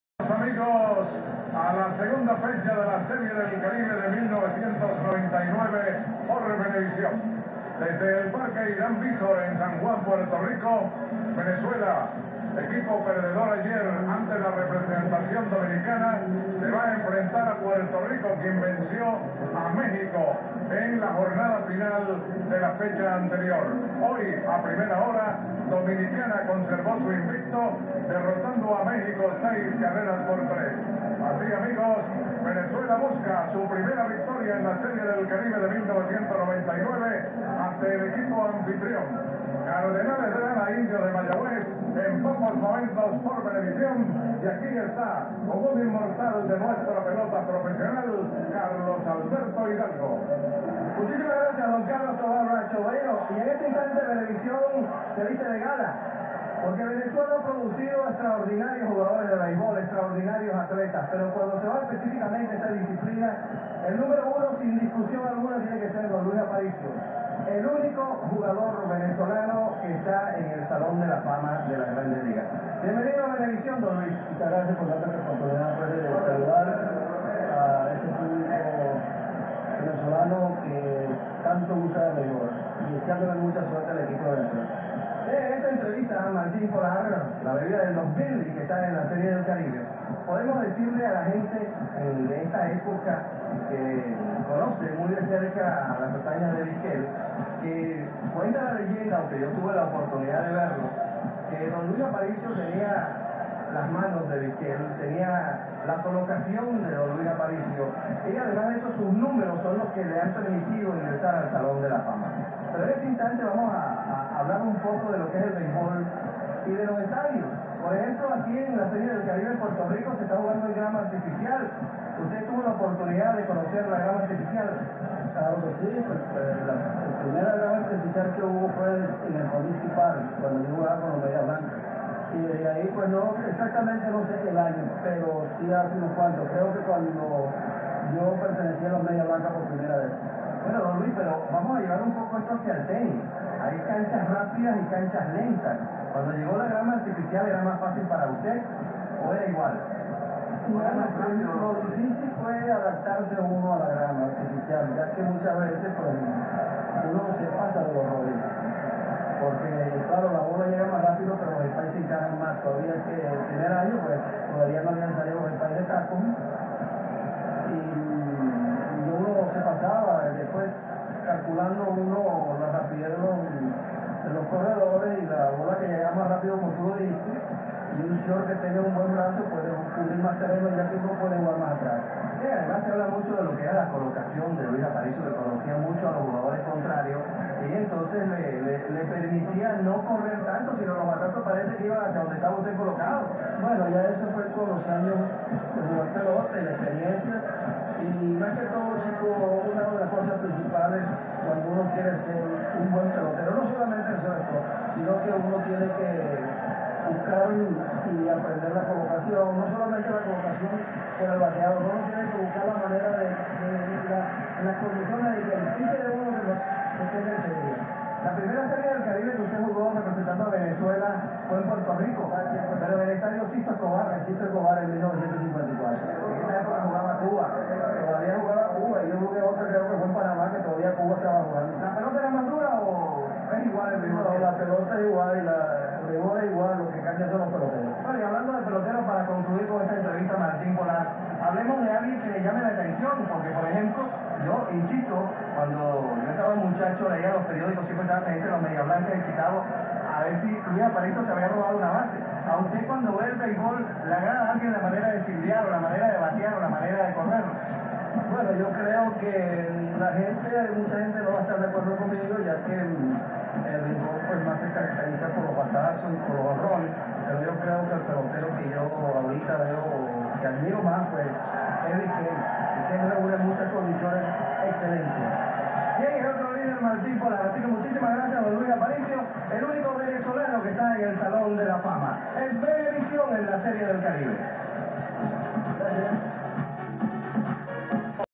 Carlos Alberto Hidaldo entrevista Luis Aparicio, en la Serie del Caribe San Juan 1999.
Carlos Alberto Hidalgo Meleán conversa con el zuliano Luis Aparicio Montiel, salón de la fama del beisbol de las grandes ligas, minutos antes del segundo juego de la serie del caribe San Juan 1999.